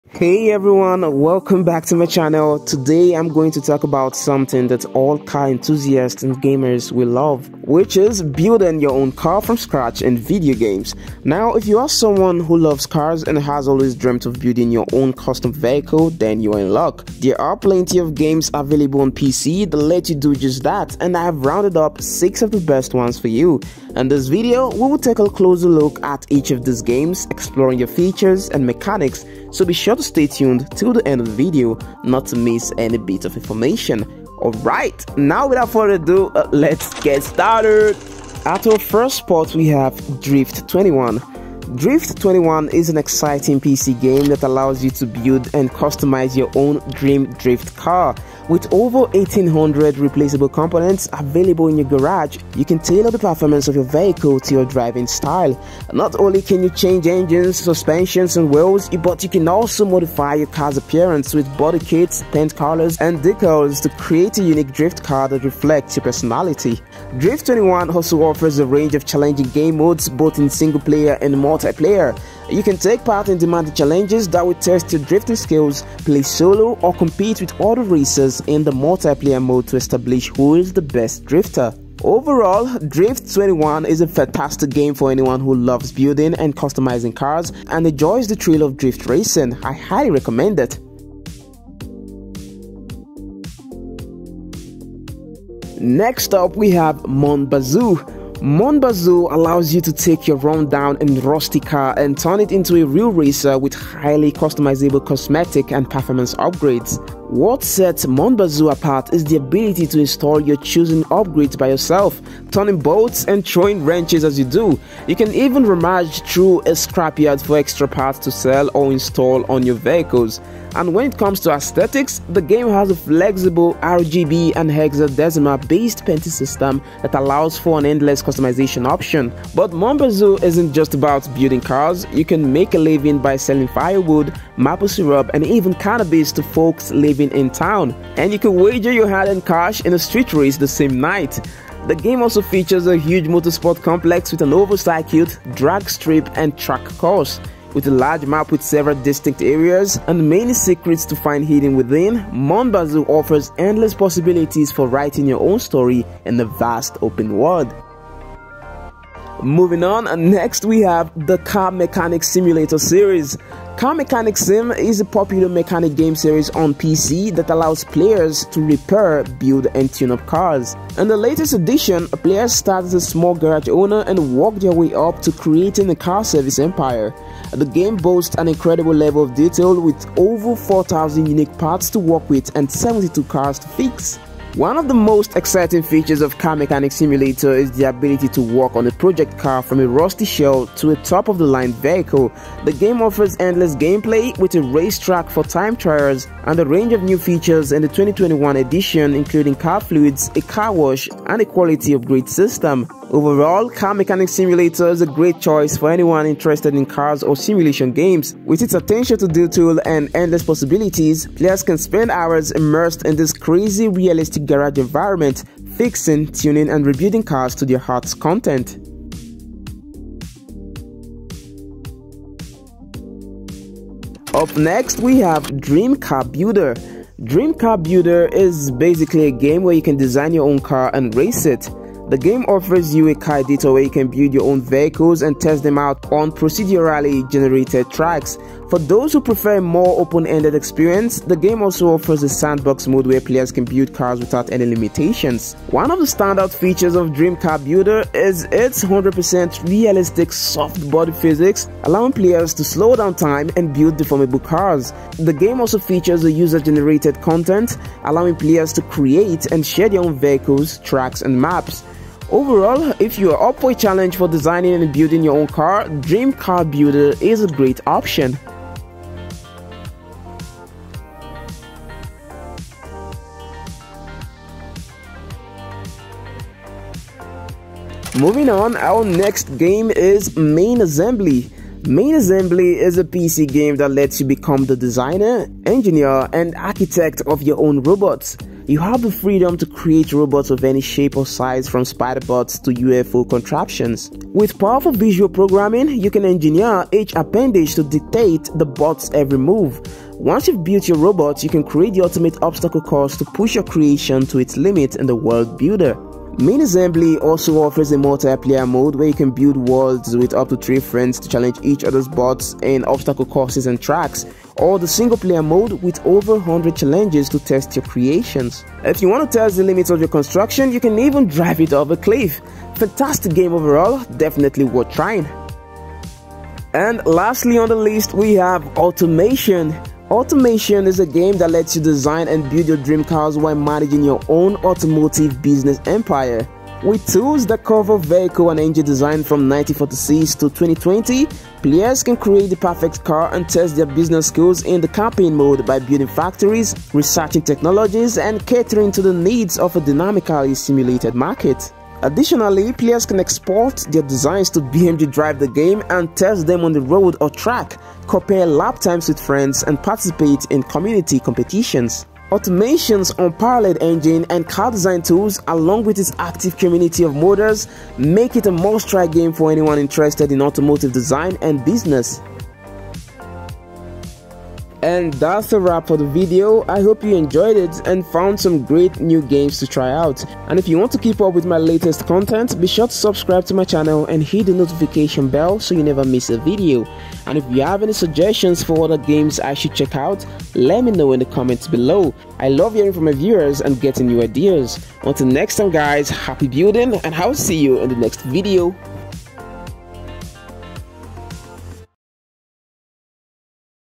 6 Games on PC That Lets you Build your own Car!
Now, If you're someone who loves cars and has always dreamt of building your own custom vehicle, then you're in luck. There are plenty of games available on PC that let you do just that, and I've rounded up six of the best ones for you.
In this video, we'll take a closer look at each of these games, exploring their features and mechanics.